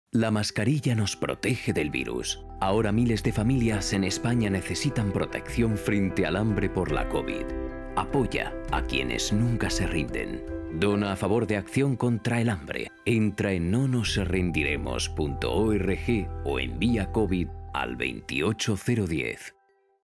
NoNosRendiremos
Esta pandemia nos ha golpeado duro. Tras la emergencia sanitaria, en Italia, en España, en Portugal... miles de personas podrían quedar excluidas, sin empleo y sin alimentos, si no hacemos nada para evitarlo.